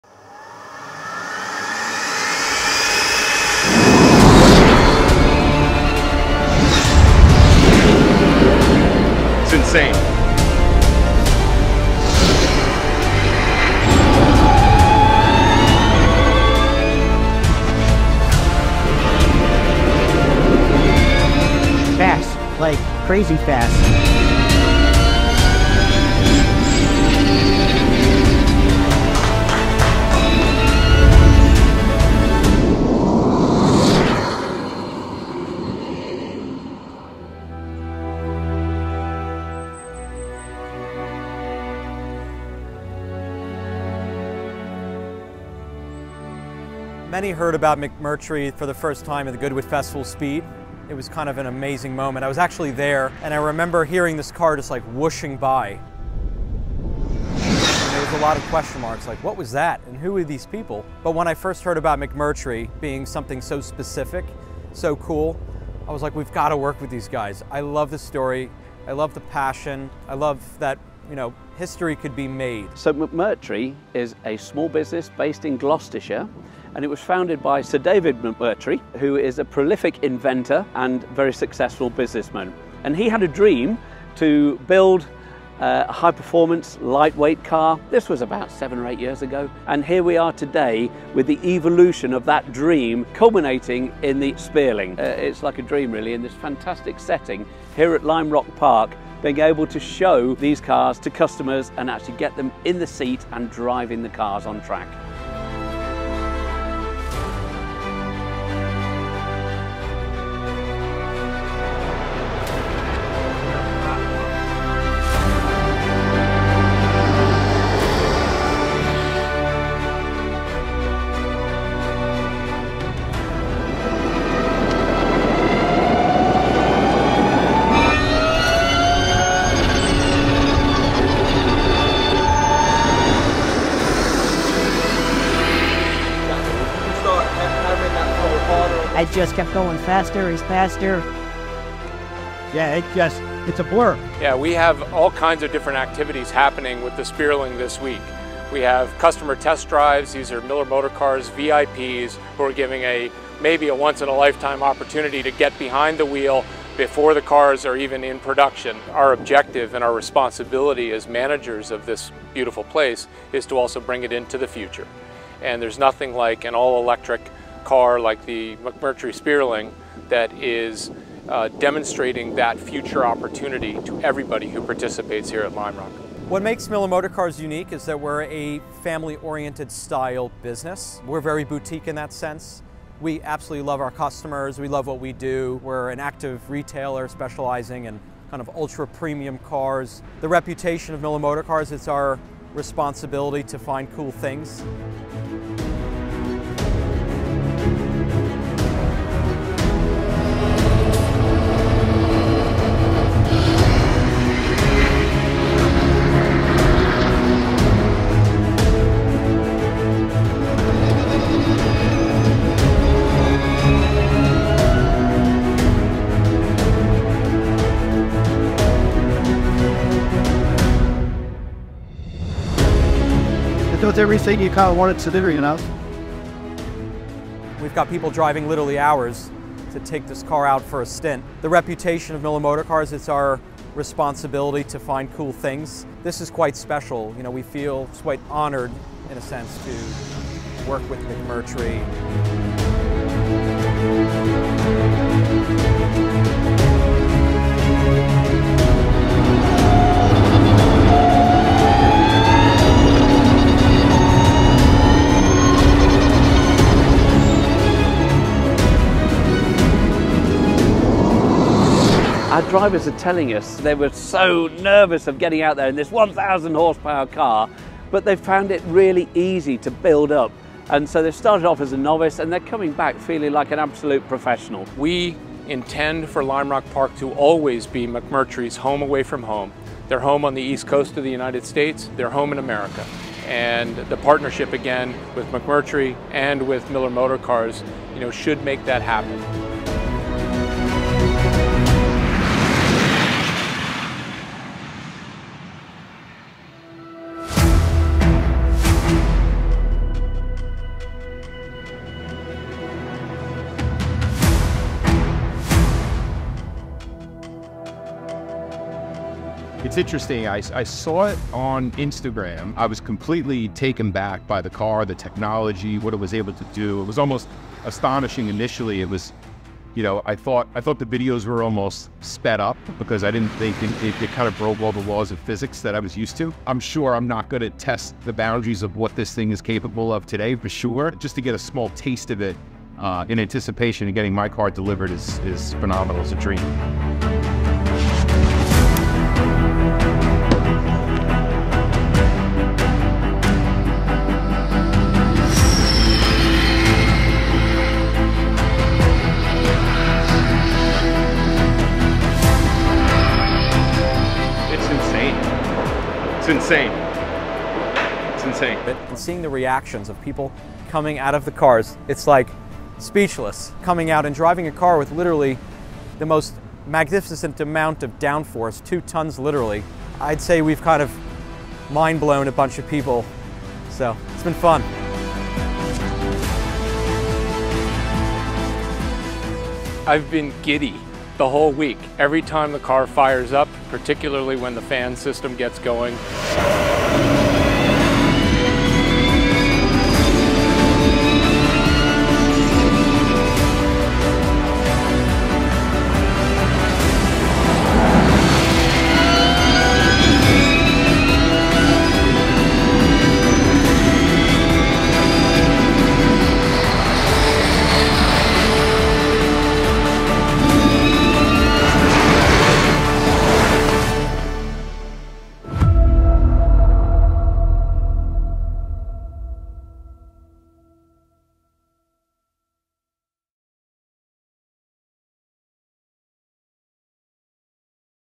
McMurtry Spéirling on track at Lime Rock Park
Miller Motorcars clients were given an exclusive opportunity to test drive the pre-production McMurtry Spéirling at the historic Lime Rock Park in Salisbury, Connecticut.

The Spéirling Pure is a fully electric, track-only car that produces 1,000 horsepower and can reach a top speed of 200mph. Acceleration numbers are mind numbing, with 0-60mph being achieved in 1.4 seconds and blows through the quarter mile in 7.97 seconds.